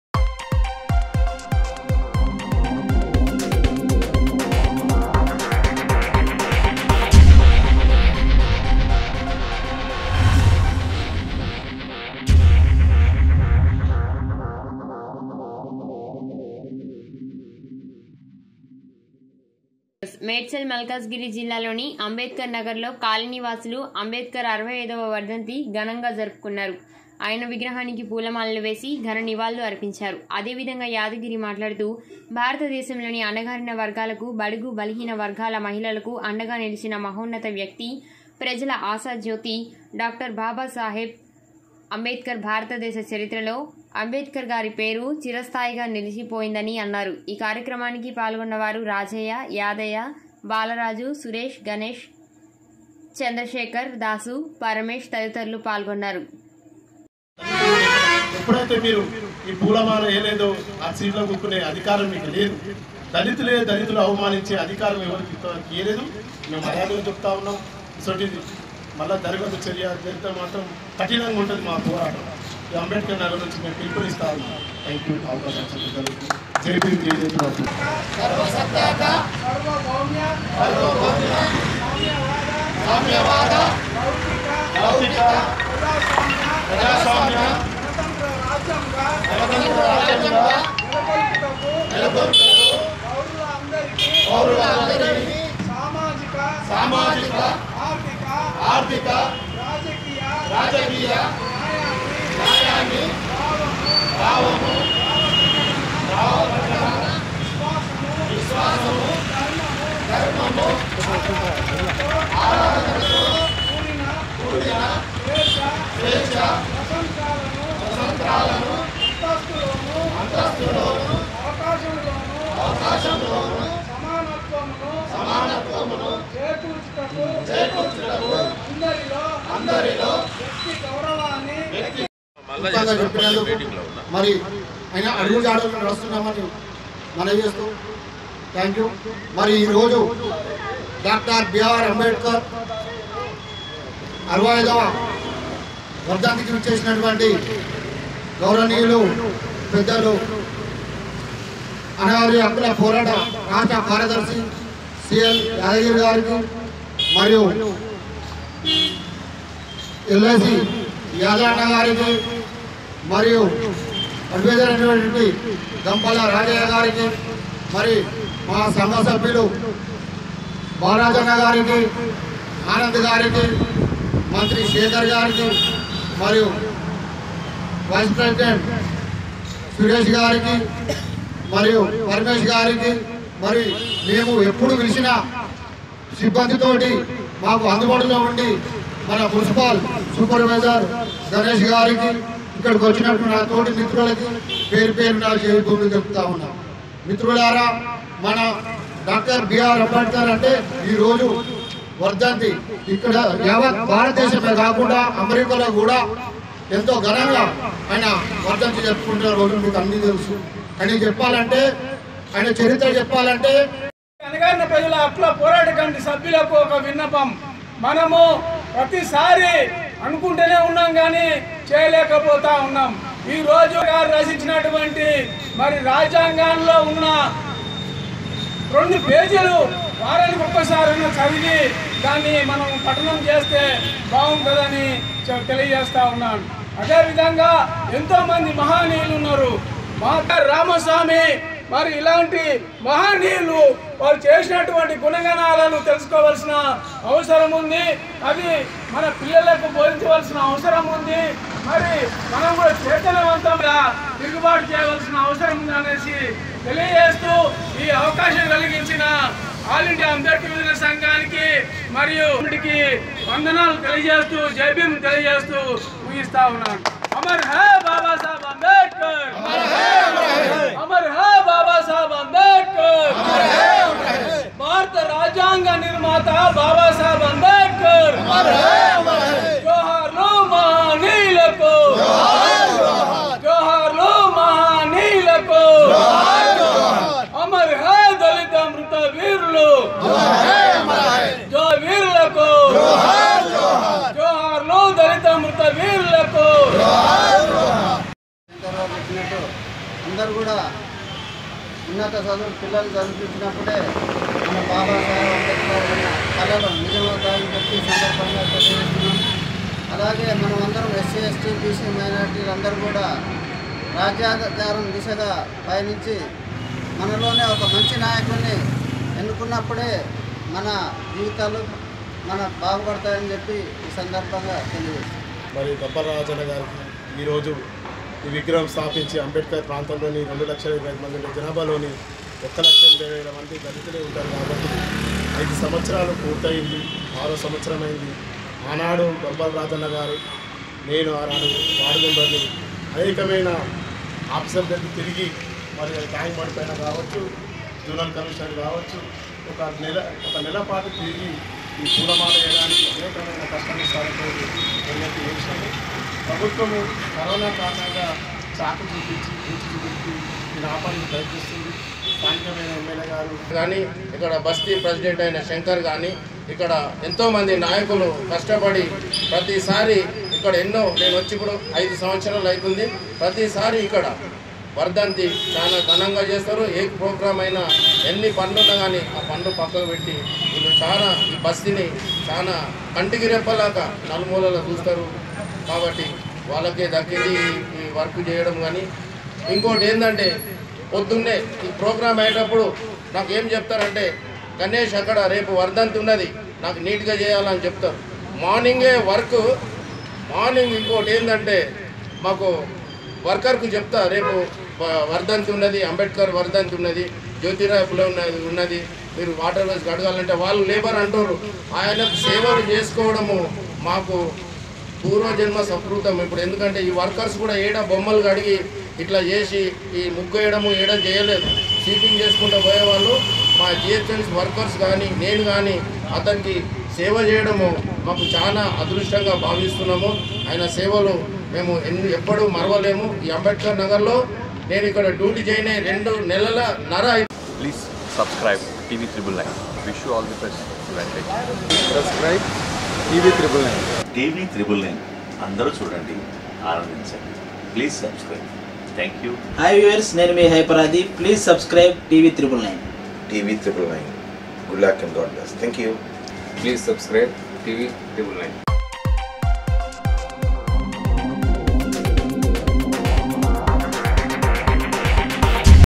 మేడ్చల్ మల్కాజిగిరి జిల్లా లోని అంబేద్కర్ నగర్ లో కాలనీవాసులు అంబేద్కర్ గారి 65 వ వర్ధంతి ఘనంగా
మేడ్చల్ మల్కాజిగిరి జిల్లా లోని అంబేద్కర్ నగర్ లో కాలనీవాసులు అంబేద్కర్ గారి  65 వ వర్ధంతి ఘనంగా జరుపుకున్నారు ఆయన విగ్రహానికి పూలమాలలు వేసి ఘన నివాళులు అర్పించారు అదేవిధంగా యాదగిరి మాట్లాడుతూ భారతదేశంలోని అణగారిన వర్గాలకు బడుగు బలహీన వర్గాల మహిళలకు అండగా నిలిచిన మహోన్నత వ్యక్తి ప్రజల ఆశాజ్యోతి డాక్టర్ బాబాసాహెబ్ అంబేద్కర్ భారతదేశ చరిత్రలో అంబేద్కర్ గారి పేరు చిరస్థాయిగా నిలిచి పోయిందని అన్నారు ఈ కార్యక్రమానికి పాల్గొన్నవారు రాజయ్య యాదయ్య బాలరాజు సురేష్ గణేశ సురేష్ చంద్ర చంద్రశేఖర్ దాసు పరమేశ్ తదితరులు పాల్గొన్నారు